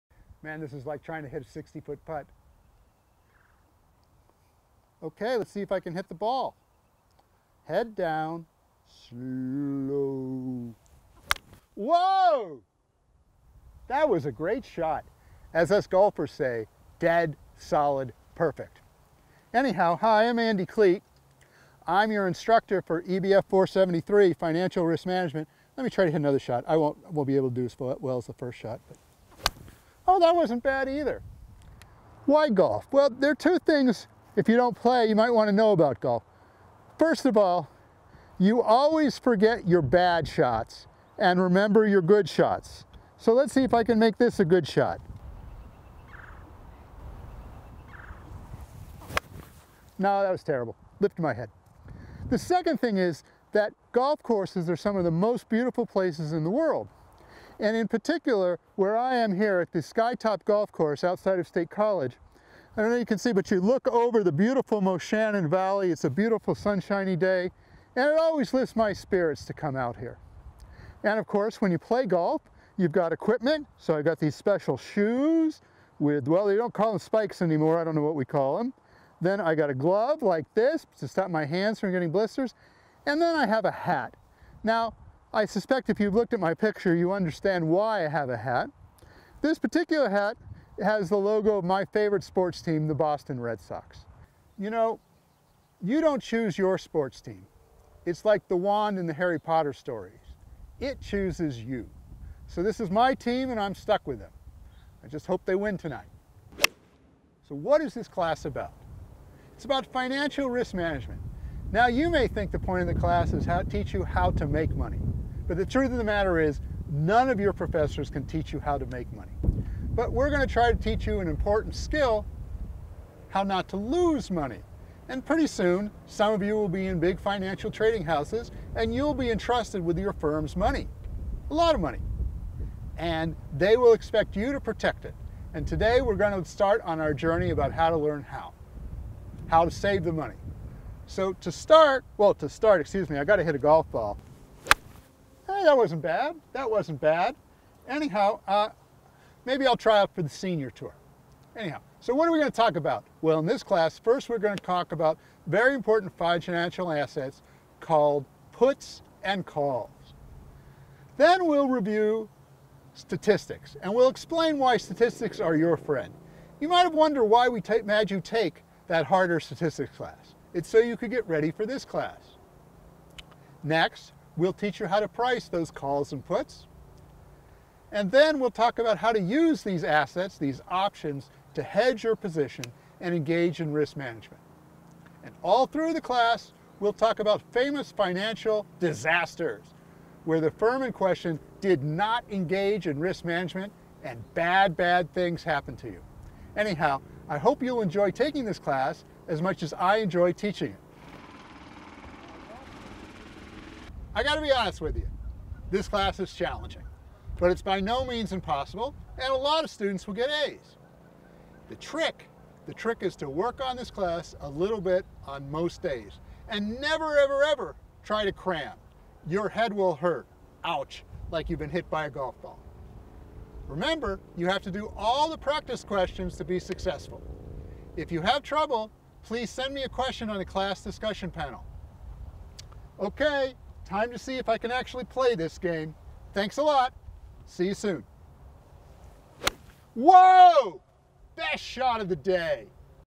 Dr Andy Kleit and Financial Risk Management
Introduction to Energy Business and Finance class 473, Financial Risk Management, at Penn State